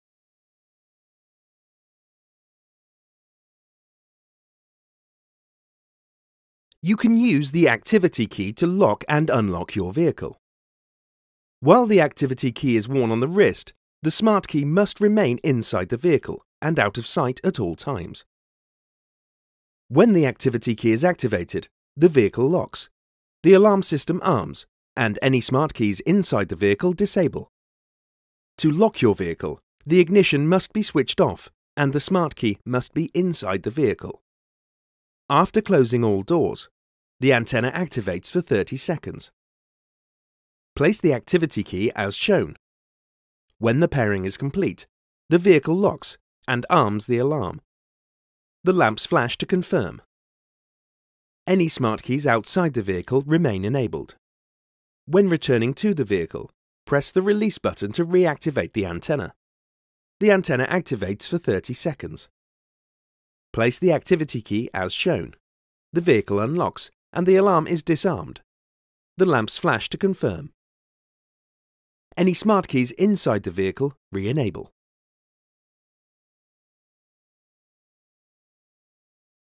2018 Range Rover Sport Activity Key
Learn how to operate the 2018 Range Rover Sport Activity Key. For more instructional information, please visit our YouTube page or our